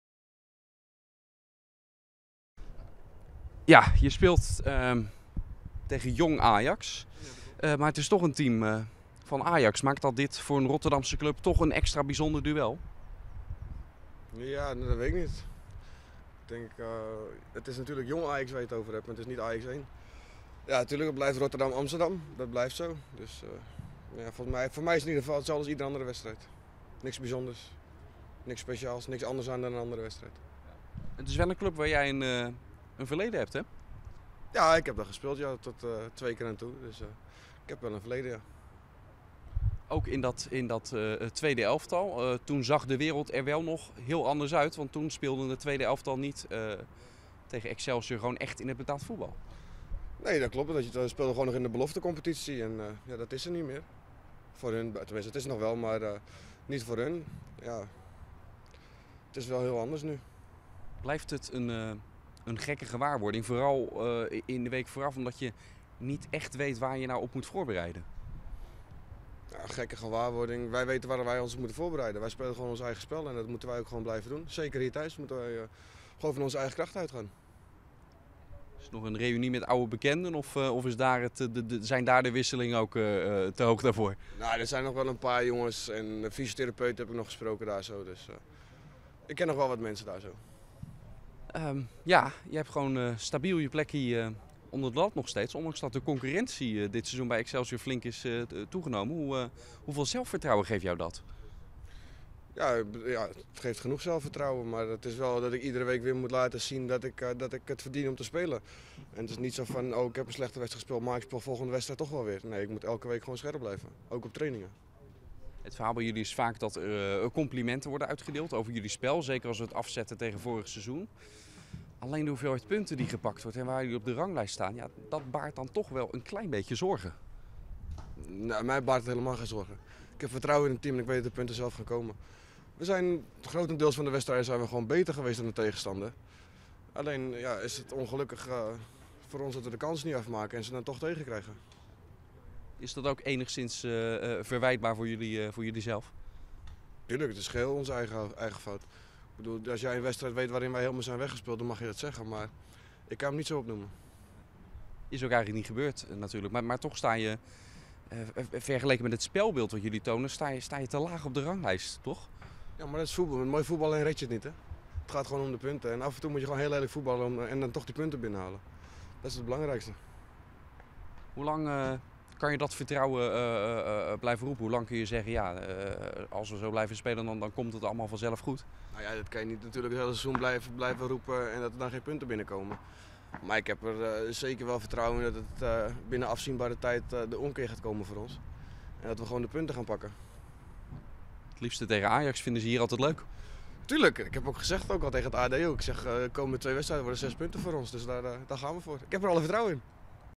Doelman Jordy Deckers over duel met Jong Ajax
Linksback Lucas Woudenberg keert terug in de basisopstelling van Excelsior, dat zondag tegen Jong Ajax speelt. De huurling van Feyenoord zat vorig weekend tegen De Graafschap nog een schorsing uit.